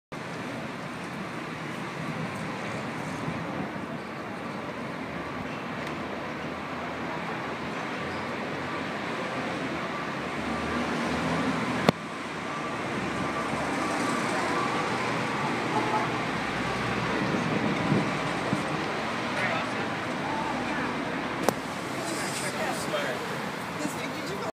Patrick Hughes In and Out. amazing art
This amazing work of art typifies the power of out of home media to engage total strangers with an aesthetic shared experience.  As the homeless man told us, "Did you look at it when you walked by?  So innovative"